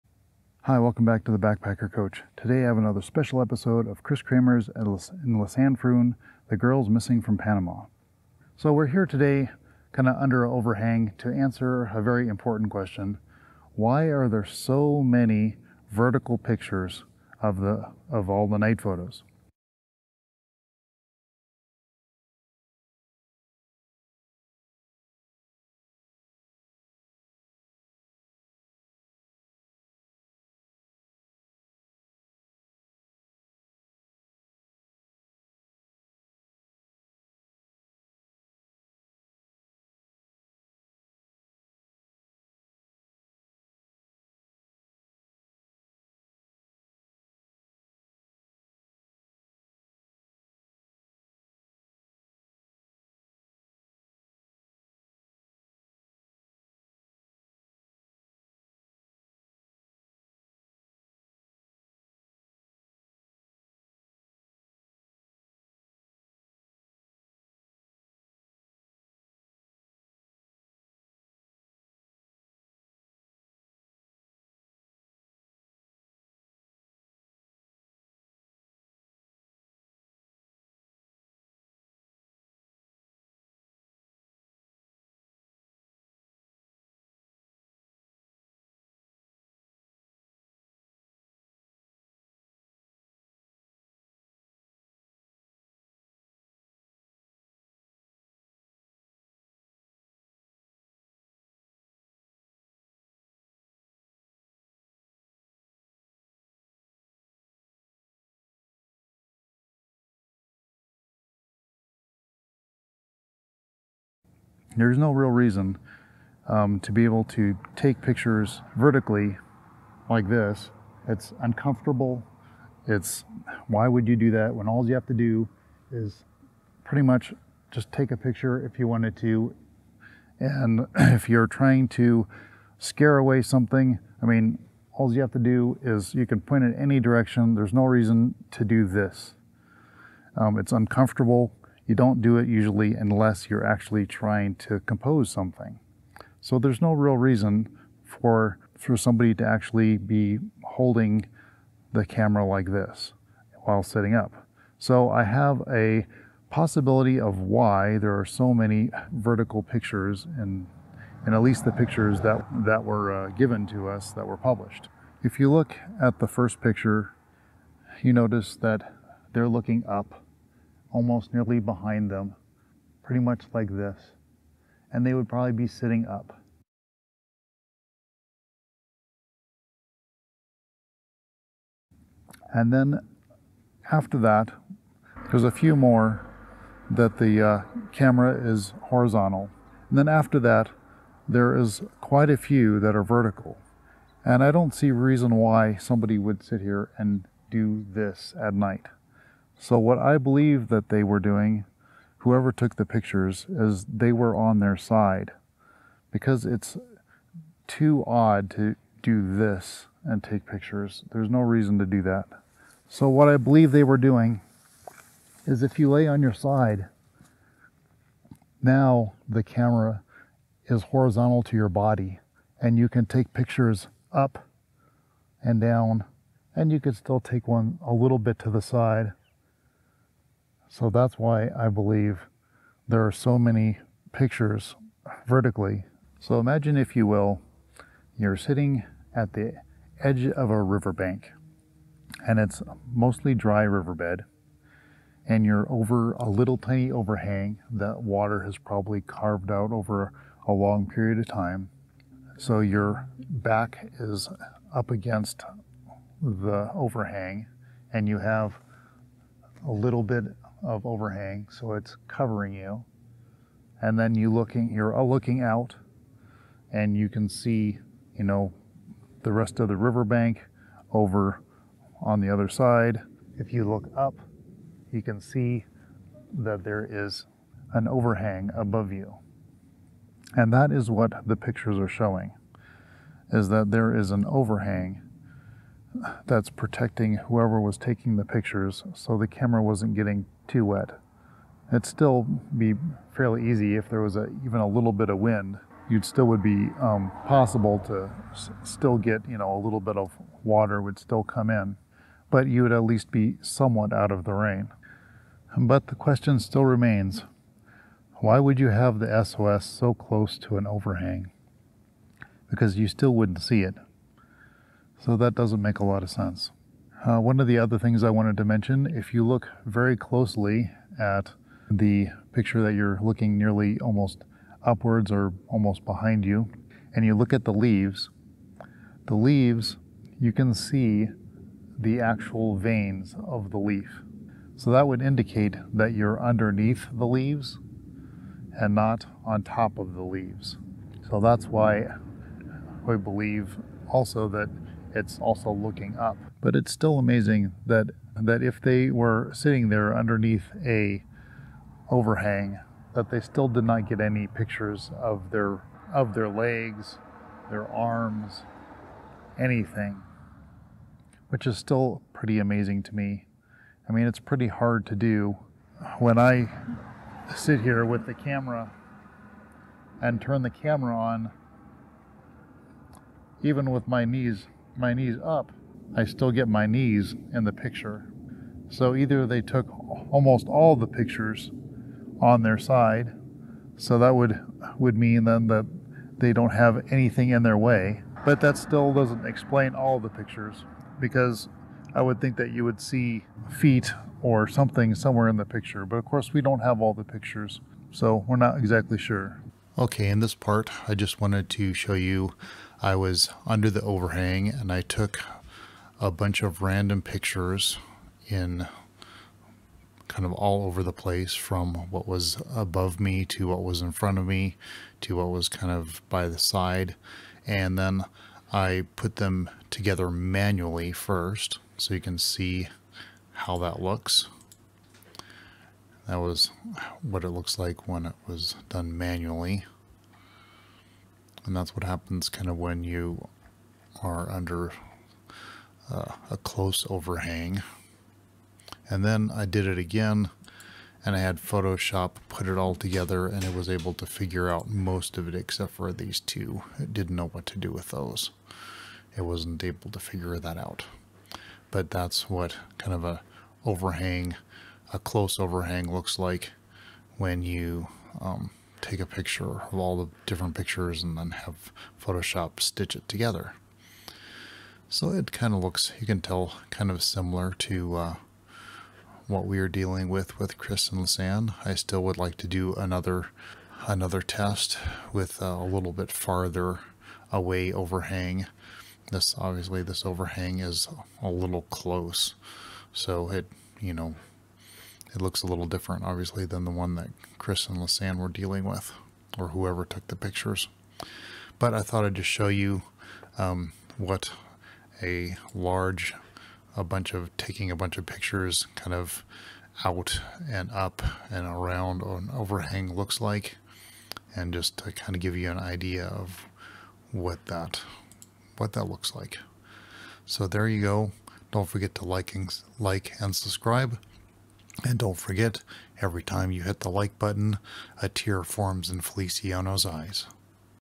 Why so many vertical night photos from Kris Kremers and Lisanne Froon?
When it comes to the night photos of Kris Kremers and Lisanne Froon.You have to wonder. Why are there so many vertical night photos? In this video we are going to find out why. Also we are going to learn how to look at the collage of night photos properly. So you get a better understanding of what you are looking at.